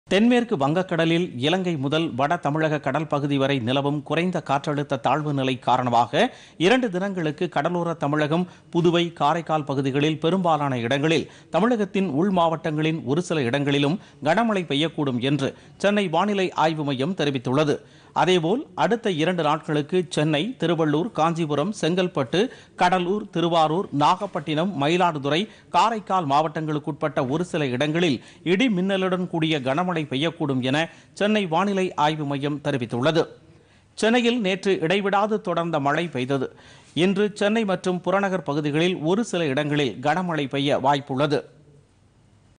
இரு தினங்களுக்கு கனமழைக்கு வாய்ப்பு
வங்கக் கடலில் குறைந்த காற்றழுத்த தாழ்வுநிலை -  தமிழகத்தின் வட கடலோர மாவட்டங்களில் இரு தினங்களுக்கு கனமழைக்கு வாய்ப்பு - சென்னை வானிலை ஆய்வு மையம்.